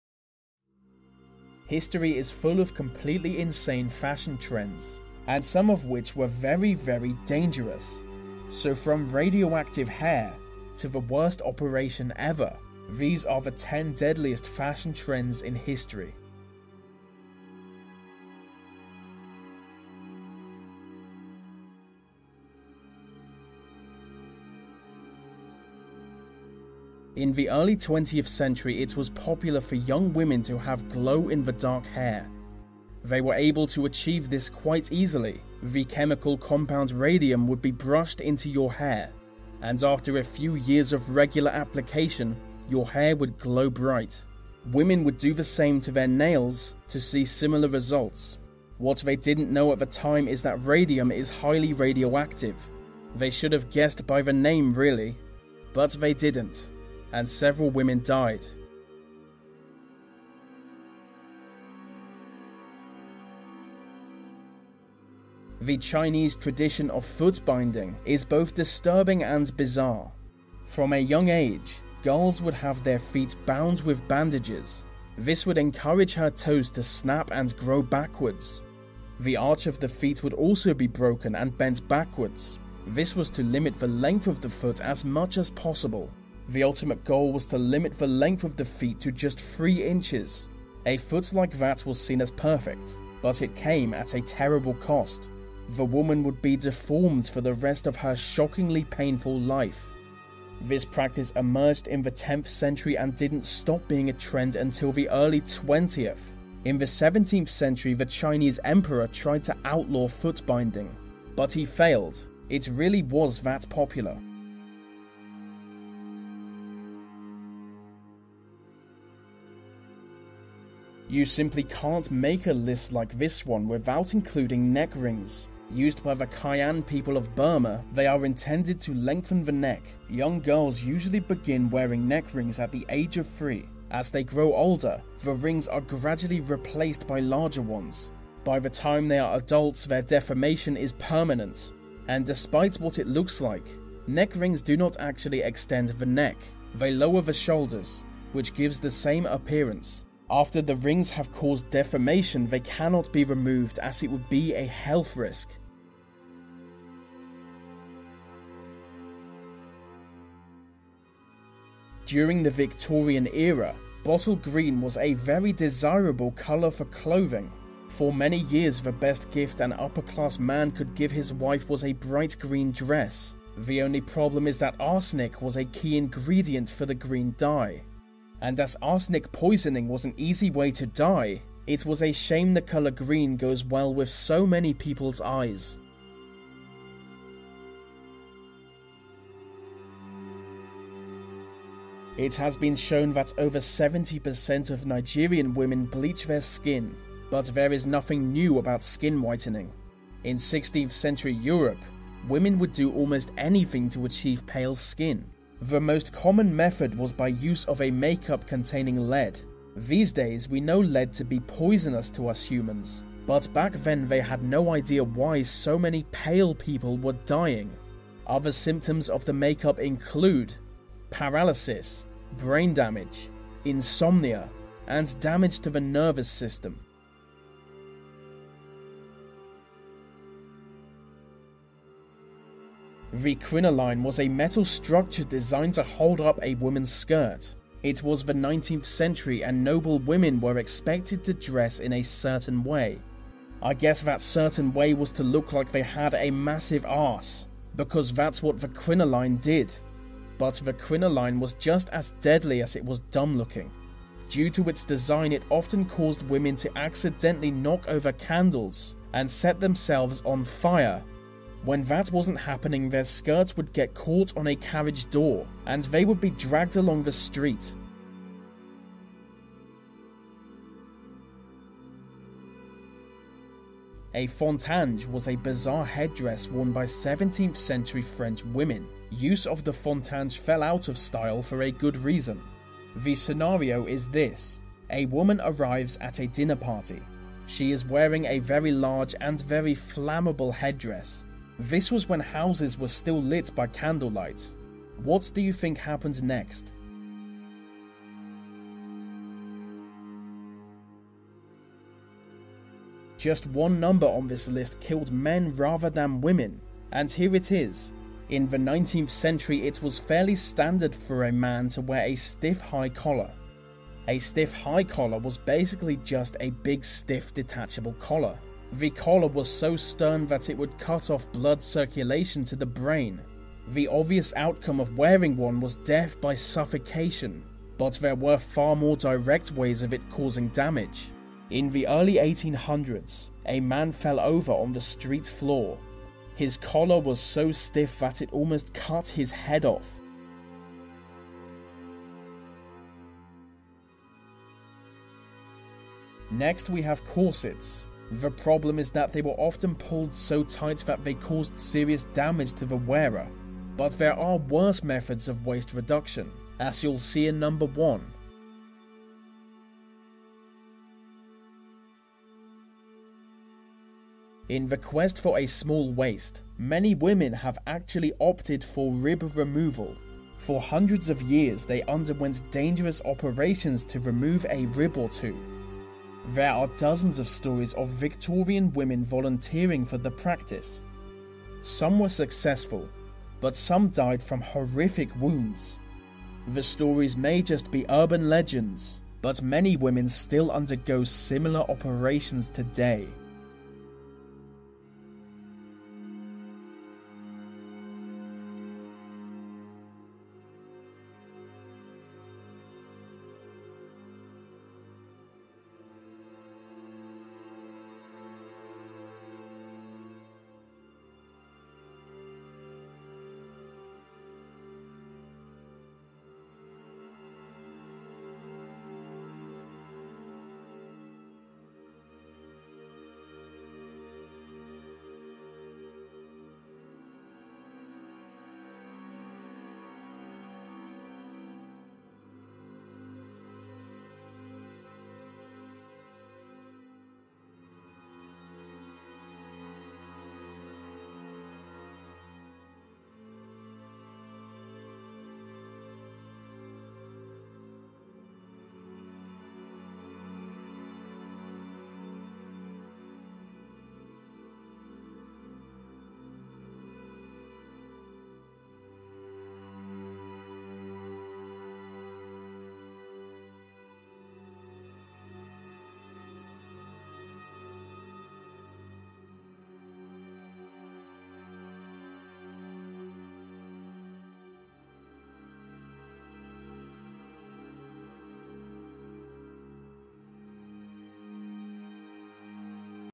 10 Deadliest Fashion Trends In History
Many women think of the old adage, “beauty is pain” everyday as they undergo painful beauty and fashion rituals, including tweezing eyebrows, waxing bikini lines and squeezing into breath-constricting skinny jeans. But some of these fashion and beauty practices from history make the act of pouring burning hot wax on sensitive skin, and mercilessly ripping it off look like a simple walk in the park. Taking a look at these excruciating beauty trends reveals how beauty standards can can vary widely in different cultures. Next time you have to lie on the bed and take a deep breath just to zip up your jeans, be thankful a servant isn’t lacing up your corset. With all of the dangerous fashion trends throughout history that lead to maiming, discomfort and even death, perhaps the answer to these dangerously restrictive fashion trends can only be found by living in nudist colonies?

Check out the video for the Top 10 deadl
iest fashion trends in history.

Check out our fashion that's bang on trend and so comfy!!